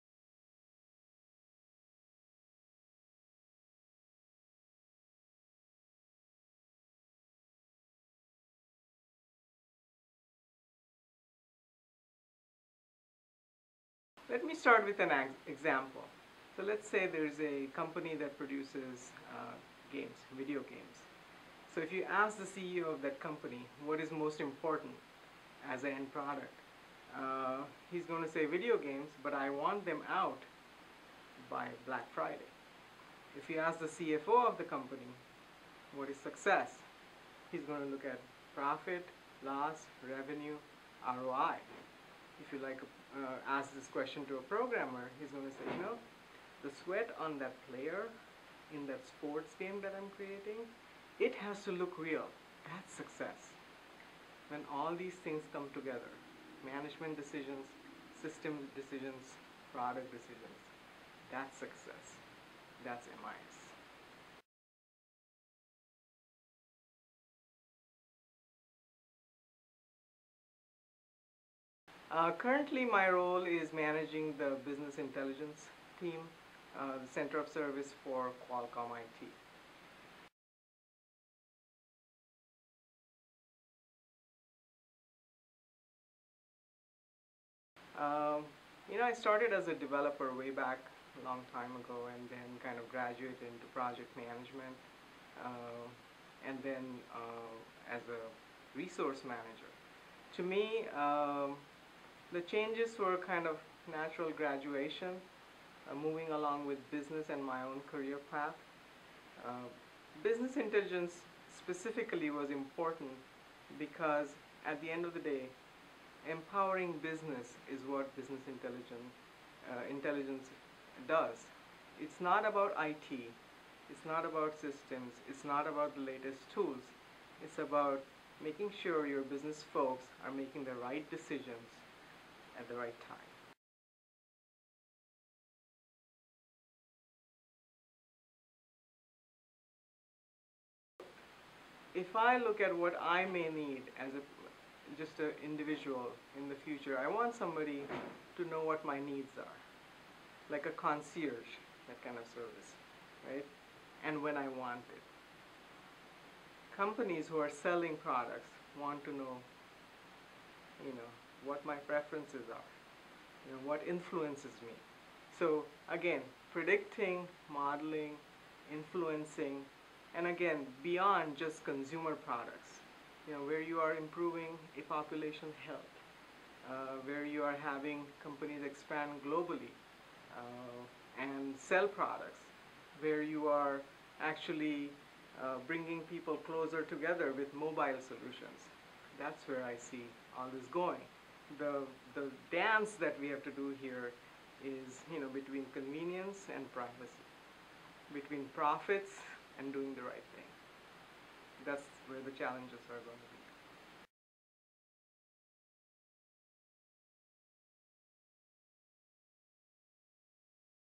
What is MIS - Pradnya Powale
Information isn't worth much if it doesn't serve a purpose.

Management Information Systems (MIS) students learn how businesses use information to improve the company's operations and how to manage various information systems so that they best serve the needs of managers, staff and customers. MIS students learn how to create systems for finding and storing data and they learn about computer databases, networks, computer security, and lots more.

Everyone who works in business, from someone who pays the bills to the person who hires and fires, uses information systems. For example, a supermarket could use a computer database to keep track of which products sell best. And a music store could use a database to sell CDs over the Internet.

During this video Prandya Powale, a executive for a large company, shares his insights as to what MIS means to him and how he uses it every day.

The MIS department at the University of Arizona is a top-five program offering quality educational programs for undergraduates, masters and doctoral students for over 38 years.